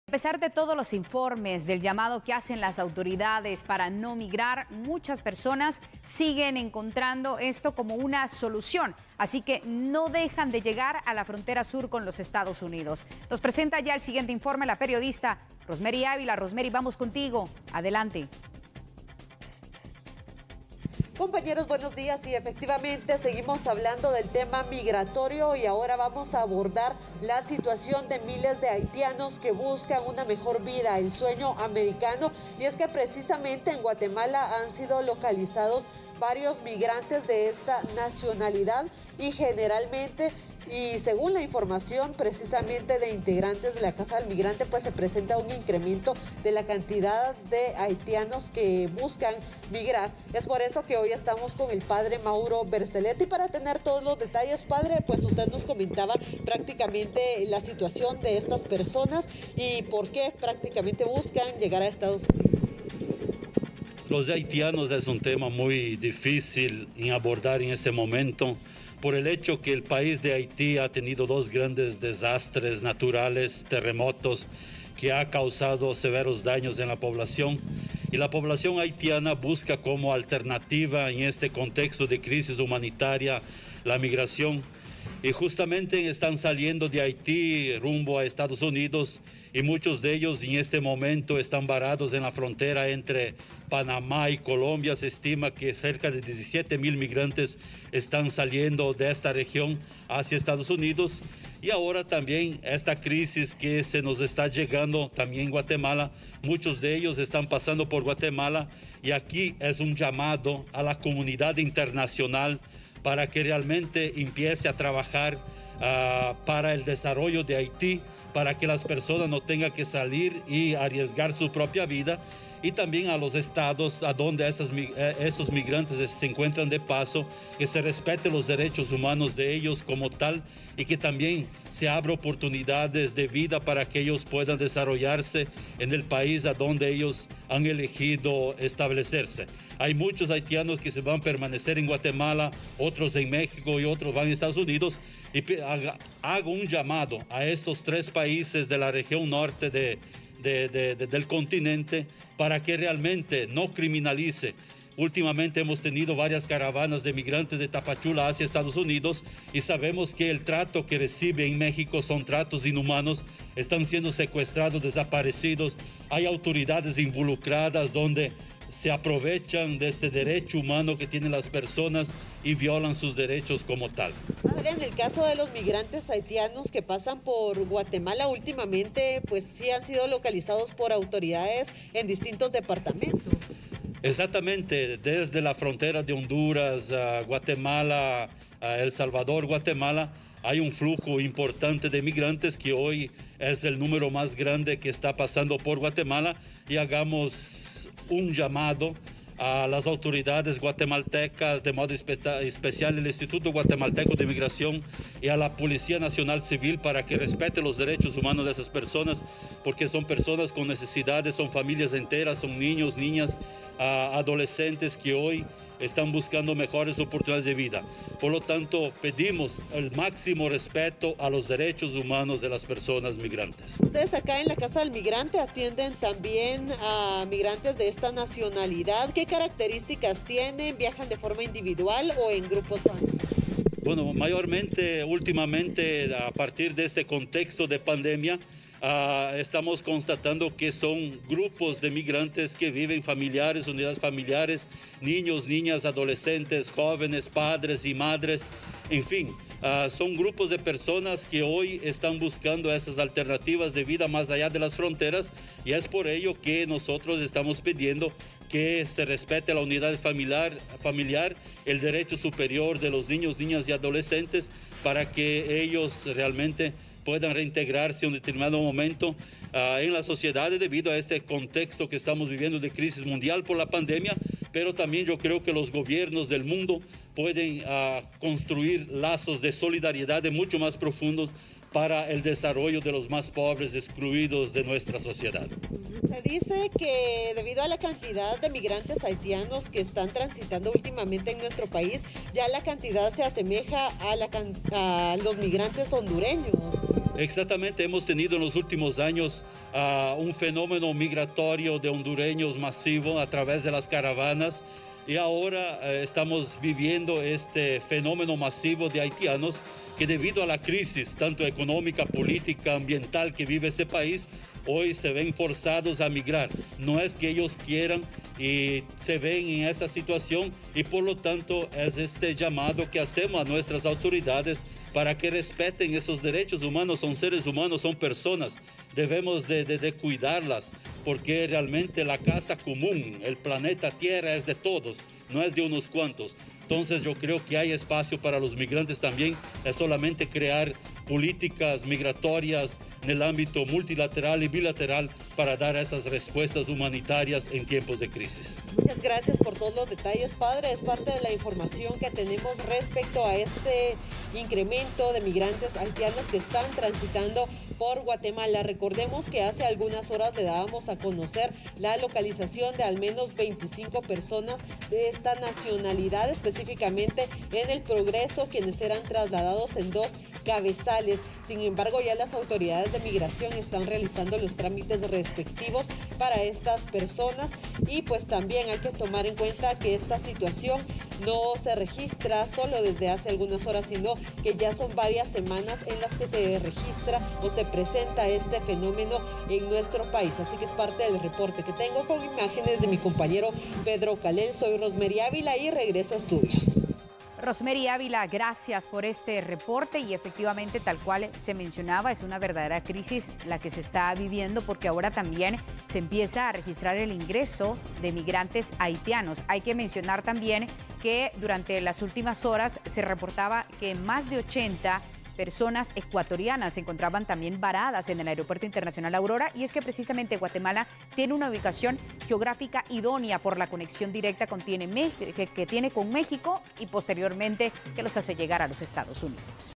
Aumenta la cantidad de migrantes haitianos
La cantidad de migrantes haitianos se ha incrementado durante las últimas semanas en su trayecto por Guatemala.

Actualmente, se reportan miles de personas de esta nacionalidad retenidas entre Colombia y Panamá.

El padre Mauro Verzeletti, vocero de la Casa del Migrante, analizó esta situación.